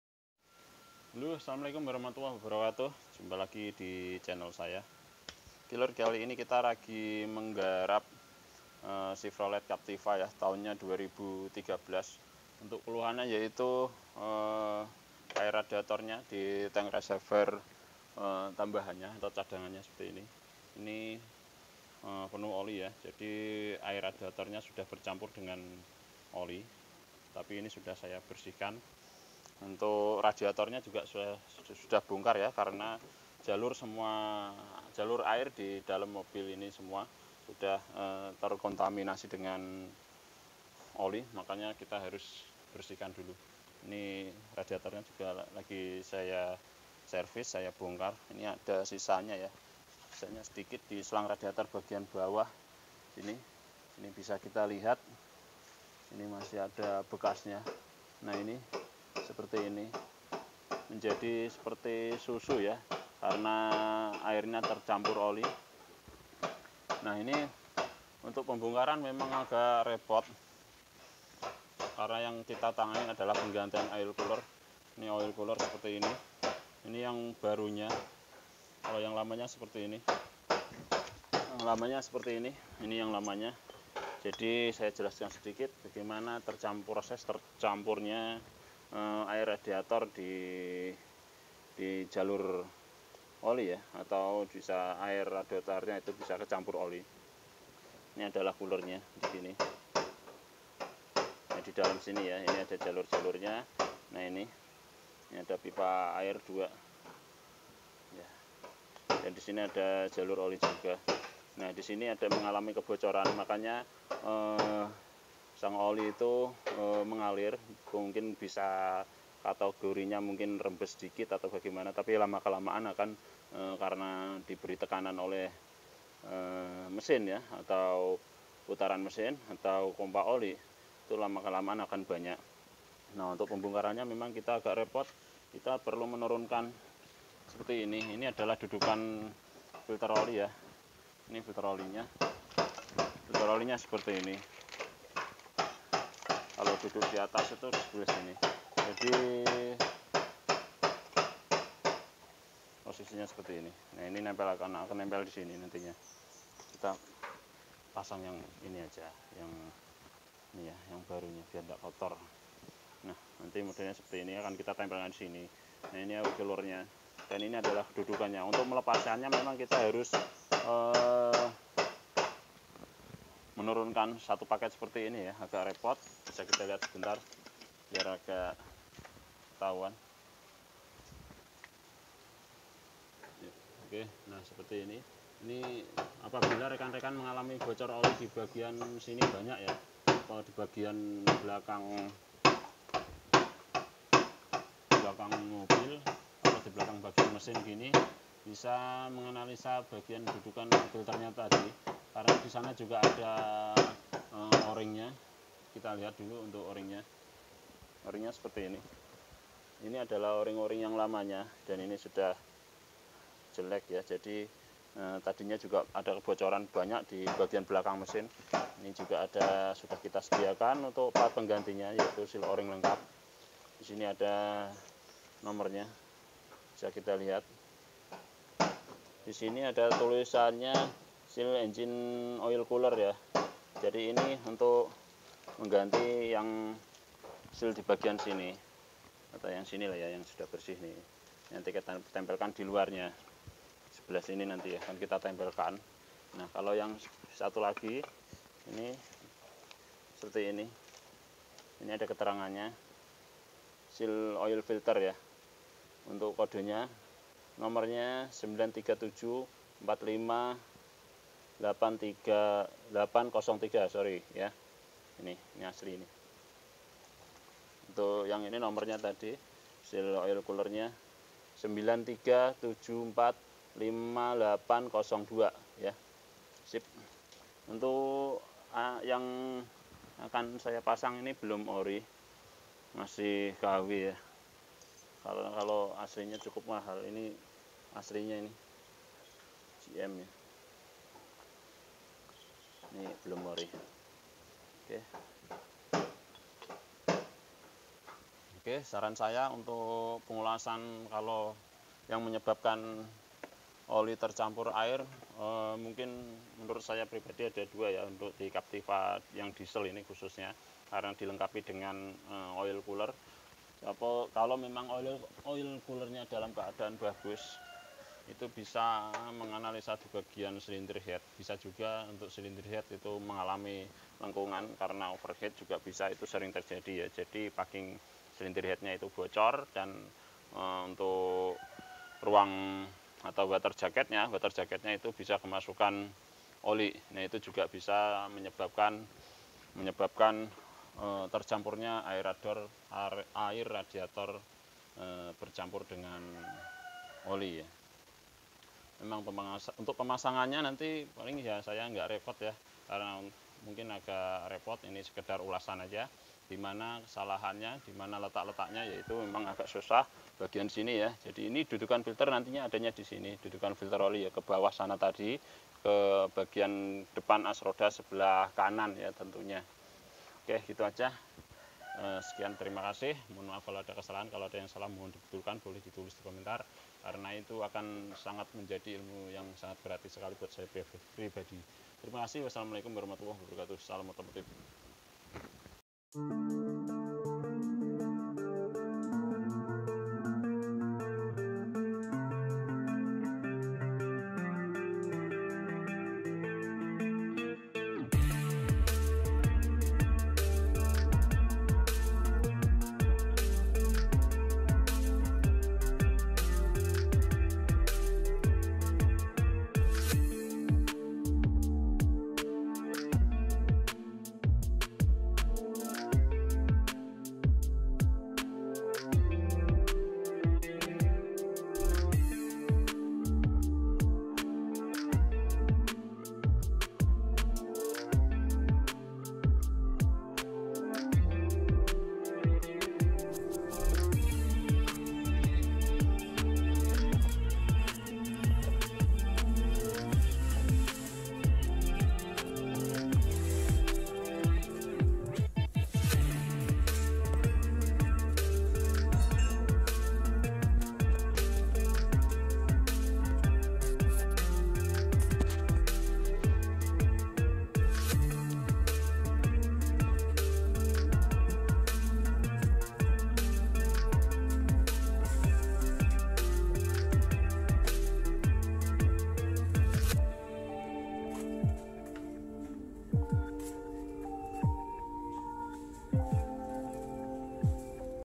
air radiator chevrolet captiva 2013 campur oli
pemasangan oil cooler captiva yang bocor mengakibatkan air radiator campur oli